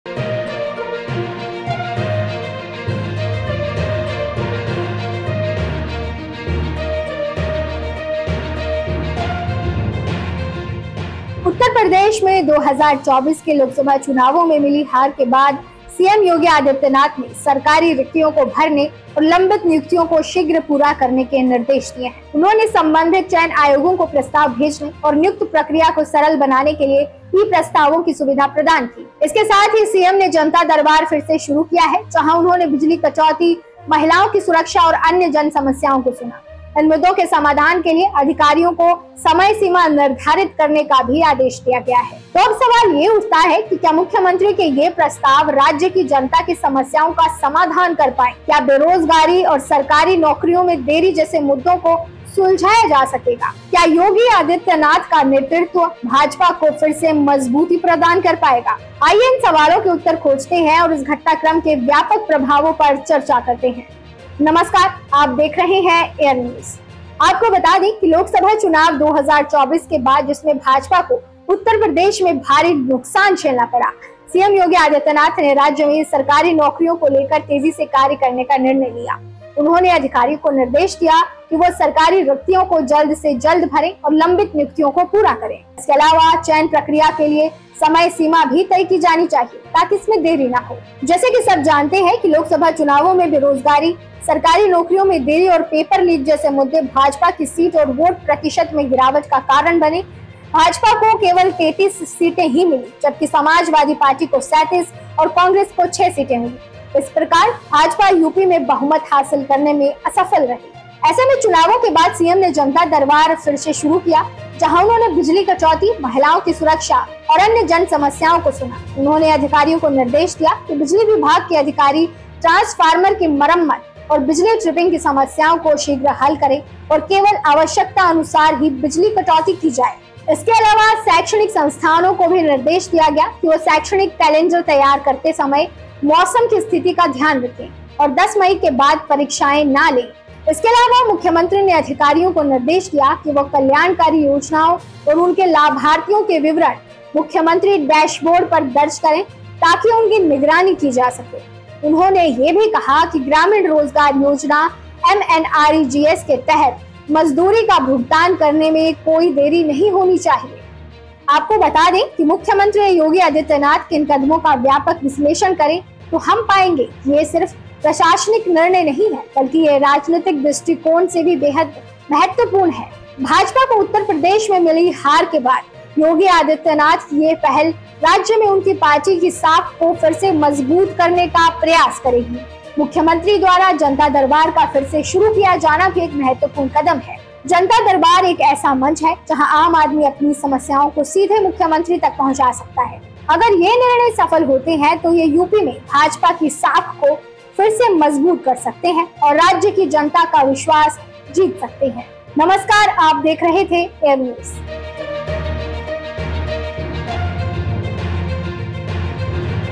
UP Chief Minister Yogi Adityanath’s Initiatives Post 2024 Elections: An In-depth Analysis
उत्तर प्रदेश में 2024 के लोकसभा चुनावों में मिली हार के बाद मुख्यमंत्री योगी आदित्यनाथ ने सरकारी रिक्तियों को भरने और लंबित नियुक्तियों को शीघ्र पूरा करने के निर्देश दिए हैं। उन्होंने संबंधित चयन आयोगों को प्रस्ताव भेजने और नियुक्ति प्रक्रिया को सरल बनाने के लिए ई-प्रस्तावों की सुविधा प्रदान की है। इसके साथ ही, मुख्यमंत्री ने जनता दरबार फिर से शुरू किया है जहां उन्होंने बिजली कटौती, महिलाओं की सुरक्षा, और अन्य जन समस्याओं को सुना। इन मुद्दों के समाधान के लिए अधिकारियों को समयसीमा निर्धारित करने का निर्देश दिया गया है।

अब सवाल यह उठता है कि क्या मुख्यमंत्री के ये प्रयास राज्य की जनता की समस्याओं का समाधान कर पाएंगे? क्या बेरोजगारी और सरकारी नौकरियों में देरी जैसे मुद्दों को सुलझाया जा सकेगा? क्या योगी आदित्यनाथ का नेतृत्व भाजपा को फिर से मजबूती प्रदान कर पाएगा? आइए, इन सवालों के उत्तर खोजते हैं और इस घटनाक्रम के व्यापक प्रभावों पर चर्चा करते हैं।

नमस्कार, आप देख रहे हैं AIRR न्यूज़।

2024 के लोकसभा चुनावों में मिली करारी हार के कुछ दिनों बाद, उत्तर प्रदेश के मुख्यमंत्री योगी आदित्यनाथ फिर से सक्रिय हो गए हैं। मुख्यमंत्री ने अधिकारियों को सरकारी रिक्तियों को भरने और लंबित नियुक्तियों को शीघ्र पूरा करने का निर्देश दिया है। उन्होंने संबंधित चयन आयोगों को प्रस्ताव भेजने के लिए कहा है और नियुक्ति प्रक्रिया को सरल बनाने के लिए ई-प्रस्तावों की सुविधा भी प्रदान की है। इसके साथ ही, मुख्यमंत्री ने जनता दरबार फिर से शुरू किया है जहां उन्होंने बिजली कटौती, महिलाओं की सुरक्षा, और अन्य जन समस्याओं को सुना। आइए, इस विषय पर विस्तार से जानकारी प्राप्त करते हैं।

आपको बता  कि लोकसभा चुनाव 2024 के बाद, जिसमें भाजपा को उत्तर प्रदेश में भारी नुक्सान झेलना पड़ा, मुख्यमंत्री योगी आदित्यनाथ ने राज्य में सरकारी नौकरियों को लेकर तेजी से कार्य करने का निर्णय लिया है। उन्होंने अधिकारियों को निर्देश दिया है कि वे सरकारी रिक्तियों को जल्द से जल्द भरें और लंबित नियुक्तियों को पूरा करें। इसके लिए उन्होंने ई-प्रस्तावों की सुविधा भी प्रदान की है जिससे नियुक्ति प्रक्रिया में तेजी लाई जा सके।

साथ ही मुख्यमंत्री ने यह भी कहा कि प्रस्ताव भेजने से पहले नियमों की गहन जांच की जाए और कोई भी विवादास्पद प्रस्ताव नहीं भेजा जाए। इसके अलावा, चयन प्रक्रिया के लिए समयसीमा भी तय की जानी चाहिए ताकि इसमें देरी न हो।

जैसा की सबको पता है कि लोकसभा चुनावों में बेरोजगारी, सरकारी नौकरियों में देरी और पेपर लीक जैसे मुद्दे भाजपा की सीट और वोट प्रतिशत में गिरावट का मुख्य कारण बने। भाजपा को केवल 33 सीटें मिलीं जबकि समाजवादी पार्टी को 37 और कांग्रेस को 6 सीटें मिलीं। इस प्रकार, भाजपा उत्तर प्रदेश में बहुमत हासिल करने में असफल रही।

ऐसे में चुनावों के बाद, मुख्यमंत्री ने जनता दरबार फिर से शुरू किया जहां उन्होंने बिजली कटौती, महिलाओं की सुरक्षा और अन्य जन समस्याओं को सुना। उन्होंने अधिकारियों को निर्देश दिया कि बिजली विभाग के अधिकारी ट्रांसफार्मर की मरम्मत और बिजली ट्रिपिंग की समस्याओं को शीघ्र हल करें और केवल आवश्यकतानुसार ही बिजली कटौती करें।

मुख्यमंत्री ने यह भी कहा कि गन्ना किसानों के पिछले सीजन के बकाया भुगतान को नए गन्ना क्रशिंग सीजन के शुरू होने से पहले कर दिया जाए। उन्होंने यह भी कहा कि भविष्य में भुगतान केवल उन्हीं किसानों को किया जाएगा जिनका रिकॉर्ड अच्छा होगा।

इसके अलावा शैक्षणिक संस्थानों को भी निर्देश दिया गया कि वे शैक्षणिक कैलेंडर तैयार करते समय मौसम की स्थिति का ध्यान रखें और 10 मई के बाद परीक्षाएं न लें। इसके अलावा, मुख्यमंत्री ने अधिकारियों को निर्देश दिया कि वे कल्याणकारी योजनाओं और उनके लाभार्थियों के विवरण मुख्यमंत्री डैशबोर्ड पर दर्ज करें ताकि उनकी निगरानी की जा सके। उन्होंने यह भी कहा कि ग्रामीण रोजगार योजना MNREGS के तहत मजदूरी का भुगतान करने में कोई देरी नहीं होनी चाहिए।

आपको बात दे की मुख्यमंत्री योगी आदित्यनाथ के इन कदमों का व्यापक विश्लेषण करें तो हम पाएंगे कि यह सिर्फ प्रशासनिक निर्णय नहीं हैं, बल्कि यह राजनीतिक दृष्टिकोण से भी महत्वपूर्ण हैं। भाजपा को उत्तर प्रदेश में मिली हार के बाद, योगी आदित्यनाथ की यह पहल राज्य में उनकी पार्टी की साख को फिर से मजबूत करने का प्रयास है। यह देखा जाना बाकी है कि ये कदम कितने कारगर साबित होंगे, लेकिन इनकी आवश्यकता को नकारा नहीं जा सकता।

क्योंकि लोकसभा चुनाव 2024 में भाजपा की हार का एक बड़ा कारण बेरोजगारी और सरकारी नौकरियों में देरी थी। उत्तर प्रदेश में युवाओं में बेरोजगारी एक गंभीर समस्या है, और सरकारी नौकरियों में देरी ने इस समस्या को और बढ़ा दिया है। पेपर लीक की घटनाओं ने भी जनता के विश्वास को कम किया है। योगी आदित्यनाथ ने इन मुद्दों को पहचानते हुए इन पर तुरंत कार्यवाही की है।

मुख्यमंत्री द्वारा जनता दरबार का फिर से शुरू किया जाना भी एक महत्वपूर्ण कदम है। जनता दरबार एक ऐसा मंच है जहां आम जनता अपनी समस्याओं को सीधे मुख्यमंत्री तक पहुंचा सकती है। इस तरह के मंच जनता के साथ सीधा संपर्क स्थापित करने में सहायक होते हैं और उनकी समस्याओं का त्वरित समाधान भी संभव होता है।

वही बिजली कटौती और महिलाओं की सुरक्षा जैसे मुद्दे भी अत्यंत महत्वपूर्ण हैं। बिजली कटौती की समस्या गर्मी के मौसम में और भी बढ़ जाती है, और महिलाओं की सुरक्षा एक संवेदनशील मुद्दा है जो जनता की प्राथमिकता में रहता है। इन मुद्दों पर योगी आदित्यनाथ का त्वरित निर्णय लेना उनके प्रशासनिक कौशल को दर्शाता है।